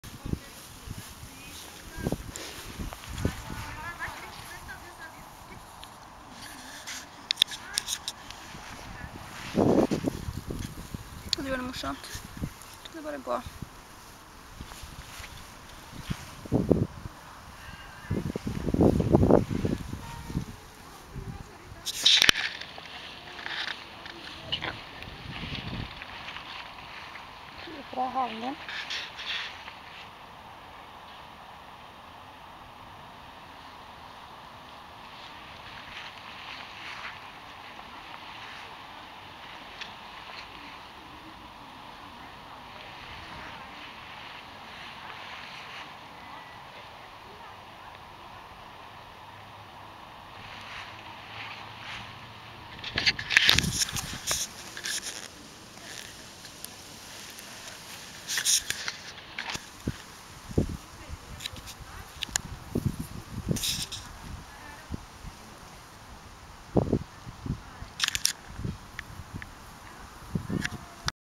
Raphael walking August 2009
This is my red eared slider turtle Raphael walking in the grass and the sun earlier today. He's pretty fast. :-)